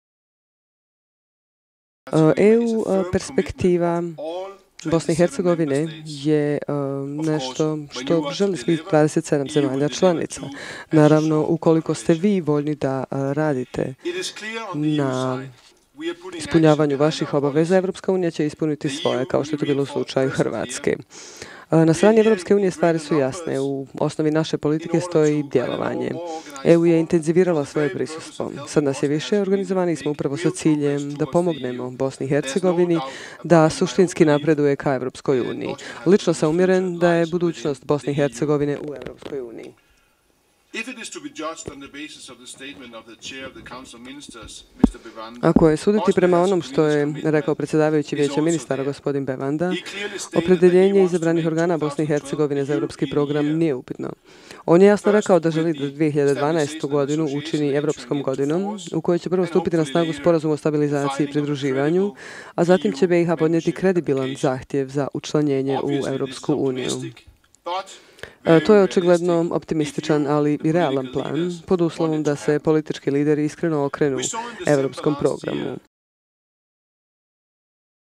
Statement by Peter Sorensen at the session of Parliament for Europe in Sarajevo
Statement by Peter Sorensen, Head of the Delegation of the European Union to BiH / European Union Special Representative in BiH at the session of Parliament for Europe held on 19th March 2012 in Sarajevo.